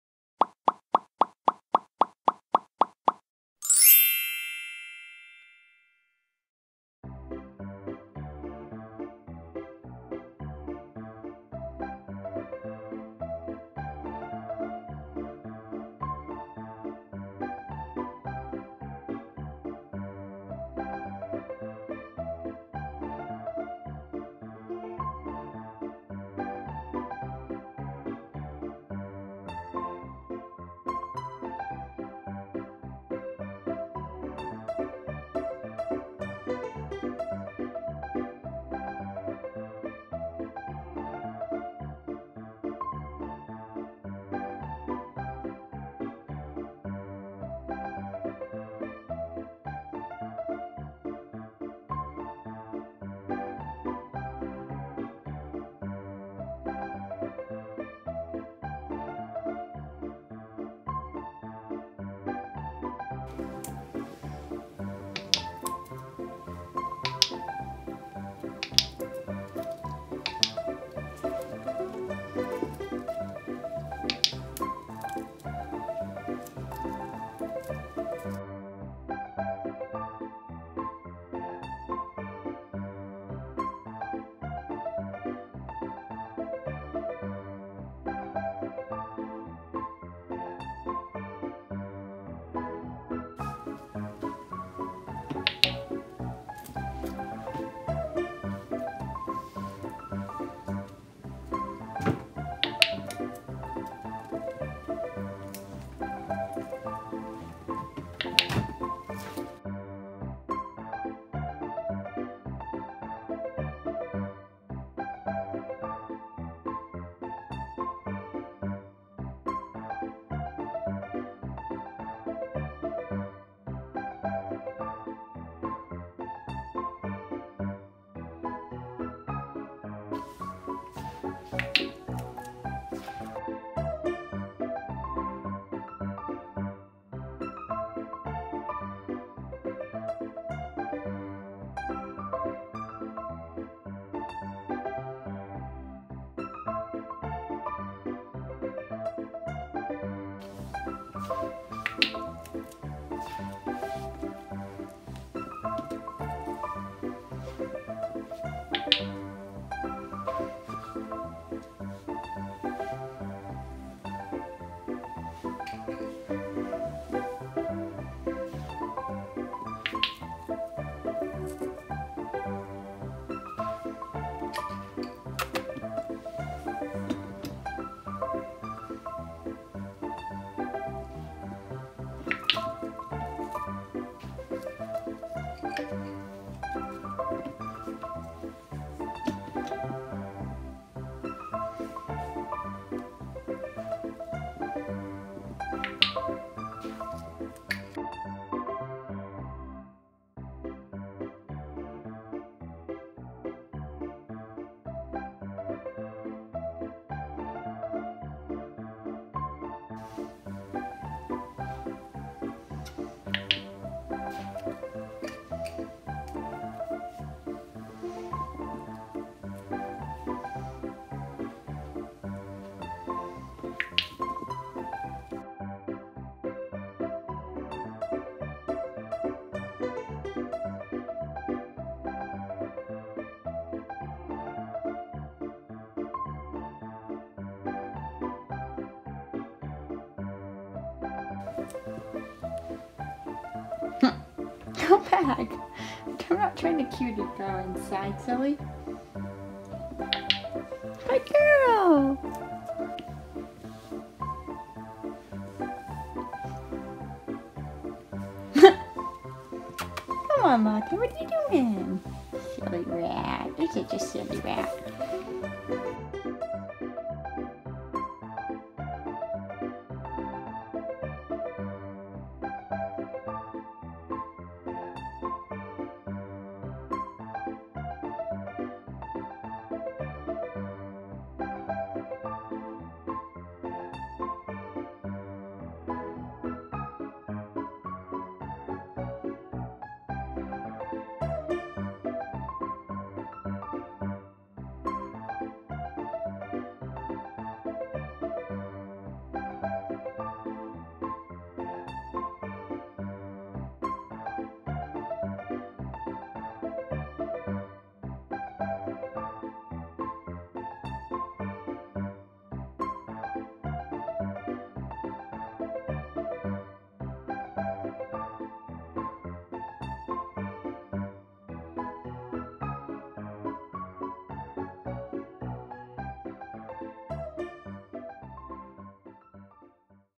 How To Train A Rat To Close A Door
In this video, Latte and Shadow demonstrate how you can train your rats to close a door on cue. If you're having trouble with this trick, try training "paws up" (link to my video on how to train this below.) Enjoy!